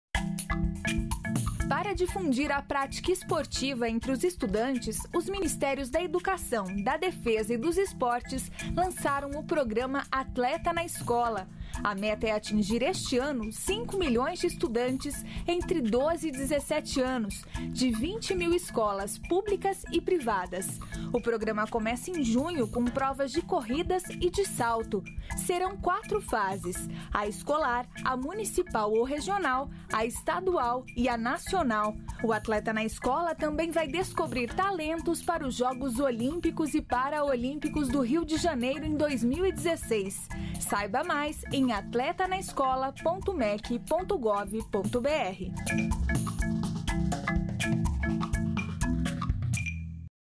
Programa incentiva prática esportiva nas escolas
FIQUE ATENTO - 14.05.13:  Para difundir a prática esportiva entre os estudantes, os ministérios da Educação, da Defesa e do Esporte lançaram o programa Atleta na Escola. A meta é atingir, este ano, cinco milhões de estudantes entre 12 e 17 anos de 20 mil escolas públicas e privadas. O programa começa em junho com provas de corrida e de salto. Serão quatro fase: escolar, municipal, estadual e  nacional. O Atleta na Escola também vai descobrir talentos para os jogos olímpicos e paraolímpicos em 2016. Mais informações em atletanaescola.mec.gov.br.